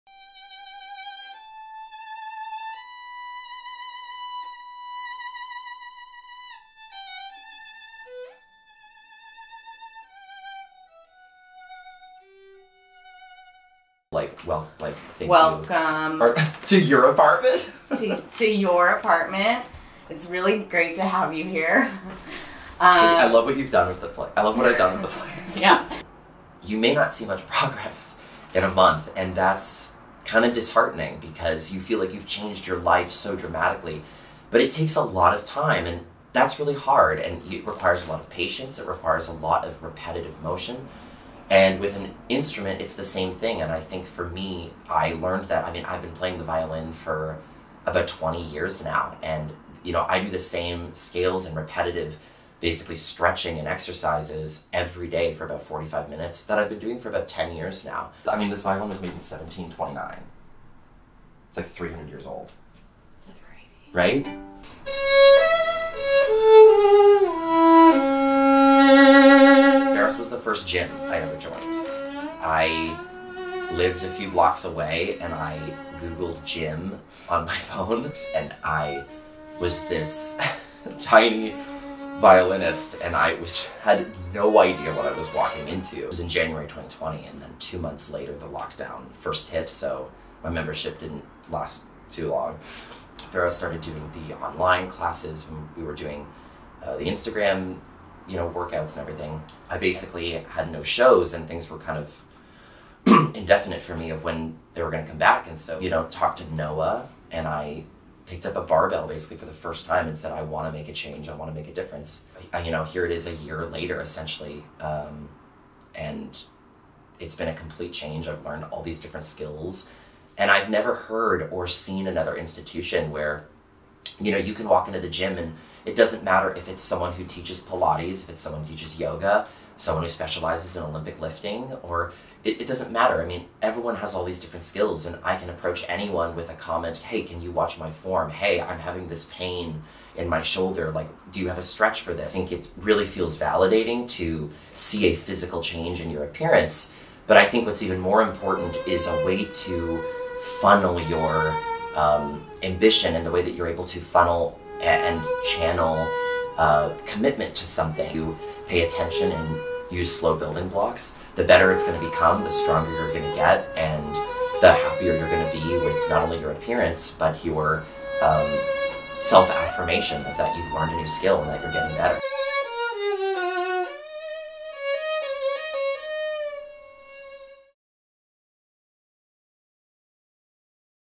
Life in High Performance with Blake Pouliot - Pharos Athletic Club
Life in High Performance with Blake Pouliot

Blake Pouliot, is a Canadian-born professional classical violinist. He studied at The Royal Conservatory of Music in Toronto and the Colburn School in Los Angeles. Blake is known for his brilliant artistry and somewhat outlandish concert clothing.

Blake is a member of Pharos Athletic Club and has gained over 22lb of muscle in a year. His journey helped propel his high-performance lifestyle and sets a perfect example of how what you do in the gym helps what you do outside of it.

If you are in Los Angeles swing by our gym at 1316 Glendale Blvd in Echo Park.